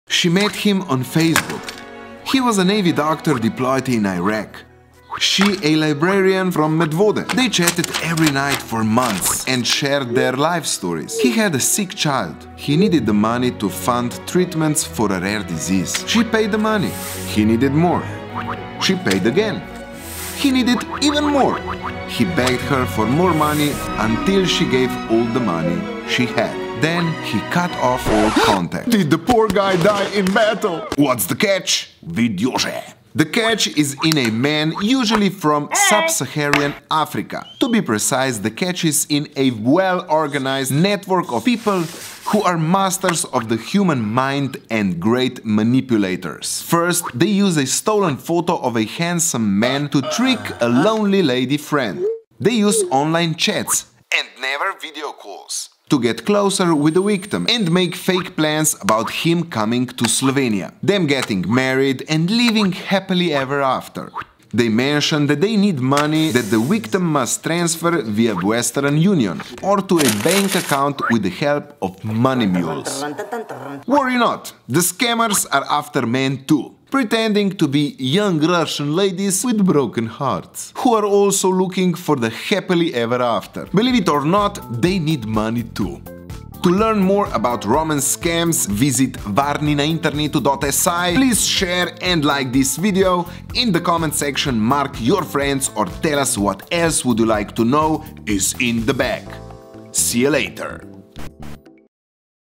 Online Romance Scam I What's The Catch? #5
When looking for love online, be careful not to find a scammer instead!

Love scam is a confidence trick often taking place on dating sites and apps, as well as popular social media sites like Facebook and Instagram. Romance scammers usually set up fake profiles using stolen photos of attractive military personnel or health professionals.

Scammers often pretend to be from Western countries travelling or working abroad (usually somewhere in the Middle East). By communicating with their victims on a regular basis over the course of many weeks or even months, they build a strong romantic relationship.

After gaining their victims' trust and affection, scammers make up a story about some sort of personal emergency and ask their victims to send them money. If the scammers don't receive the money, they become more persistent, whereas if they do - they end up asking for more.

Although a lot of these online romance scams are aimed at middle-aged women, men can fall victim too! Online dating and love scams cheat people out of millions of dollars every year, so next time you end up looking for love online - make sure your lover is not a scammer!

Watch the entire "What's The Catch" series to learn more about the workings behind some of the most popular scams.

*in Slovenian